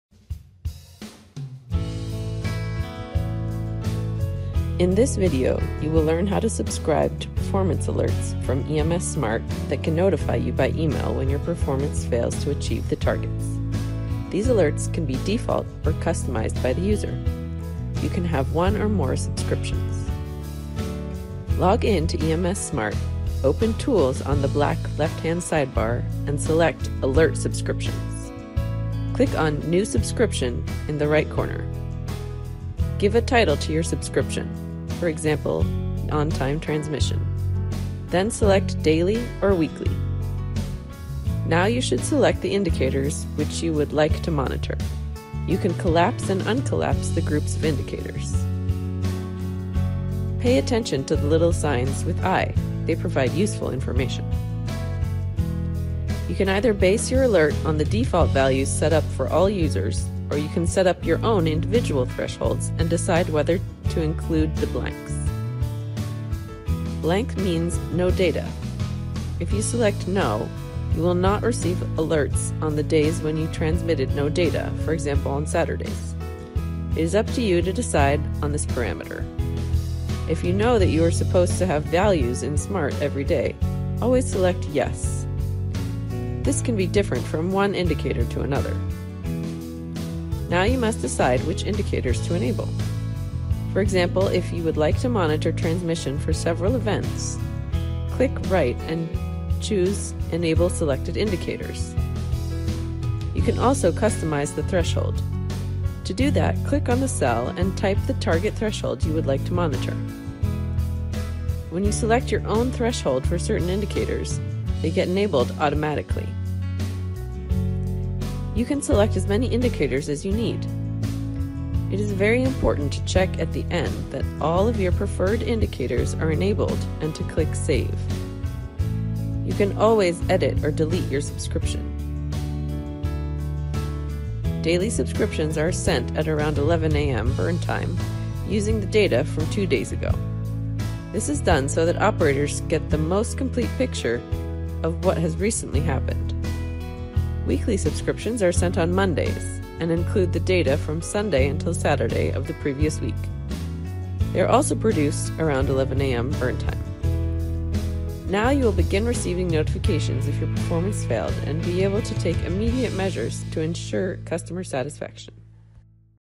EMS SMART - alert subscription
Find out how to:
- define frequency and activation status
- define targets
- create more subscriptions
- update current subscriptions.